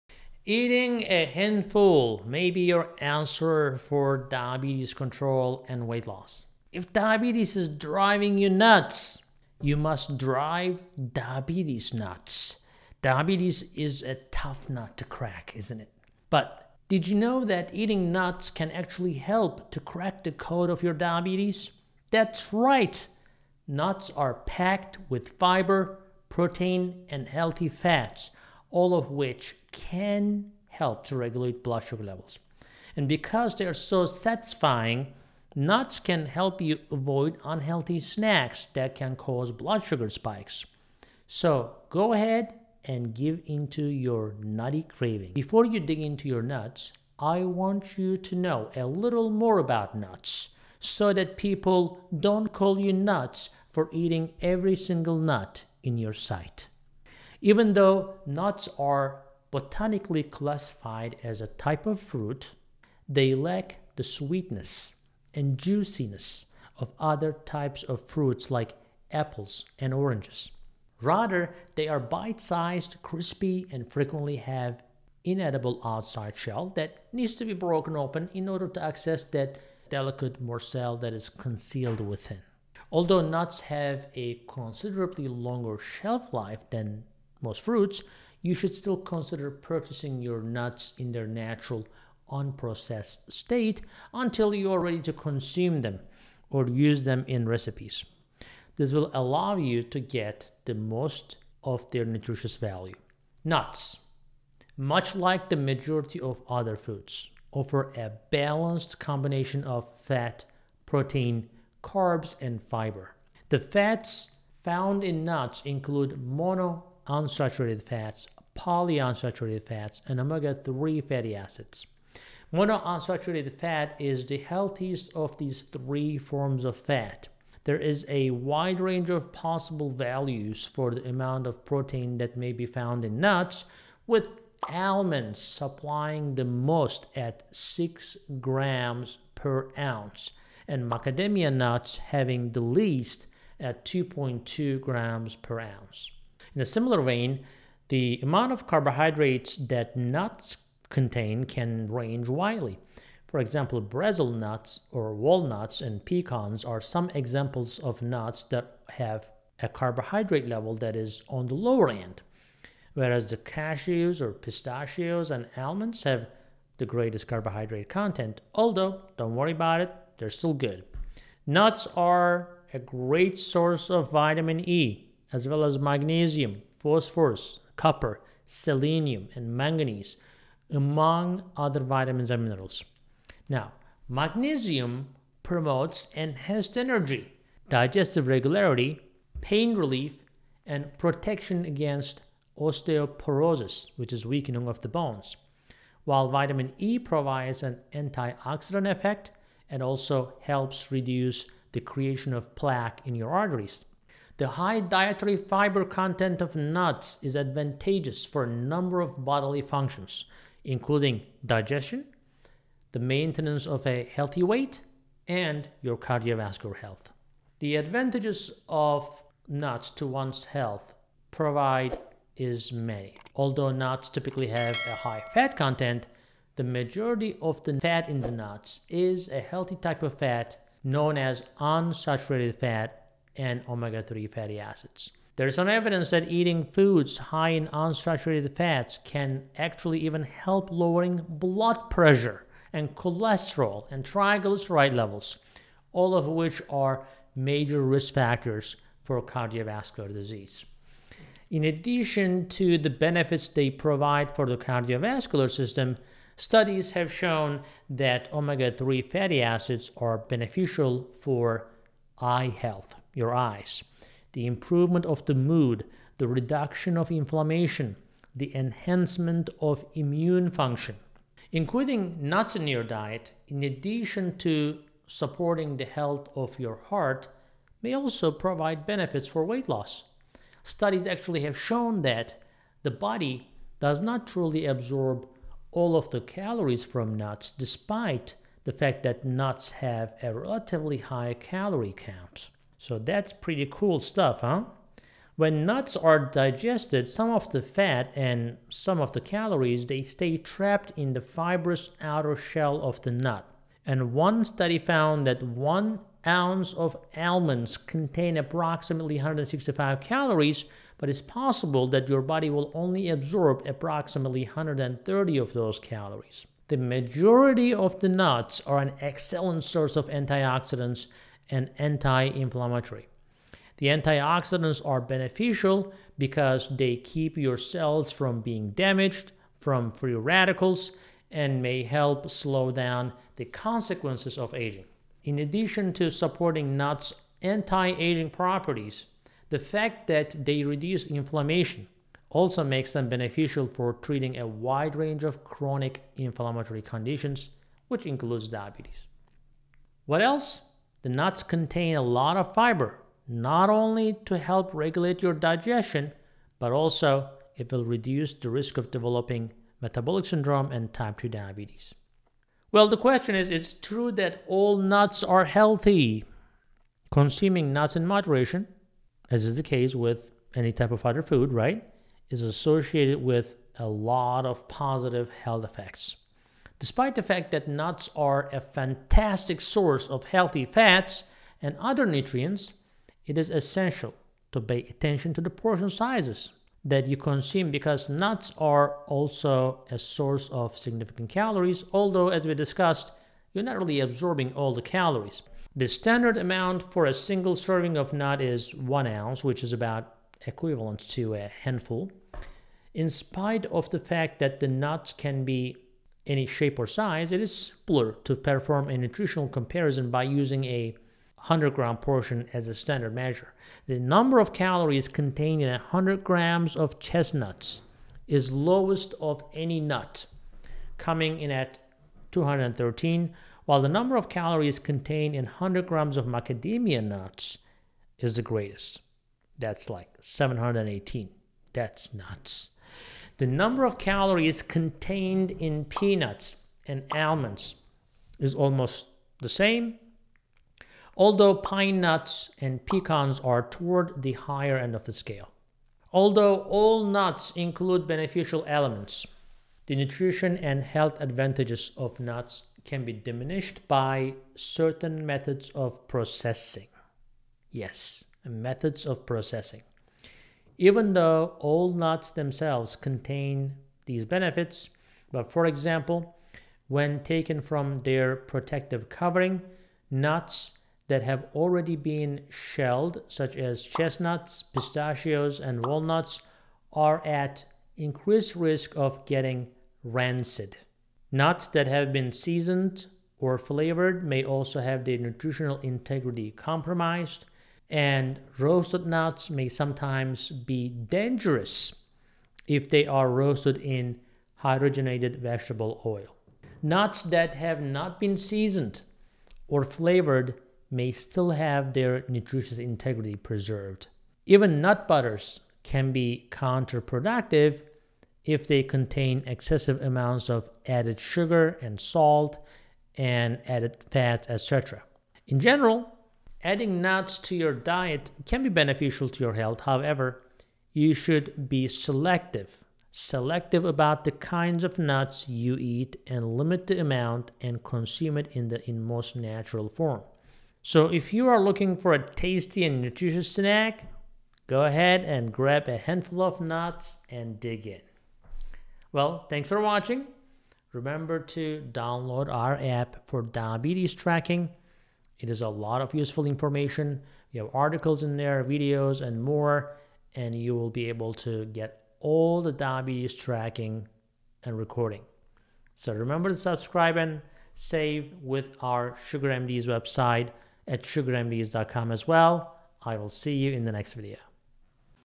Eat These Nuts To Make Glucose Normal Fast!
Followings are Dr. Ergin recommended alternatives:
1. METAFAST - New Science of Blood Sugar

2. BetaBeat - GIANT In Blood Sugar!

3. Okinawa Flat Belly Tonic

Nuts are superfood for diabetic people because they are low in carbs and high in protein, fiber, and healthy fat. In this video, Dr. Ergin will tell us why nuts should be a part of your daily diabetic diet.

Video Credit: SugarMD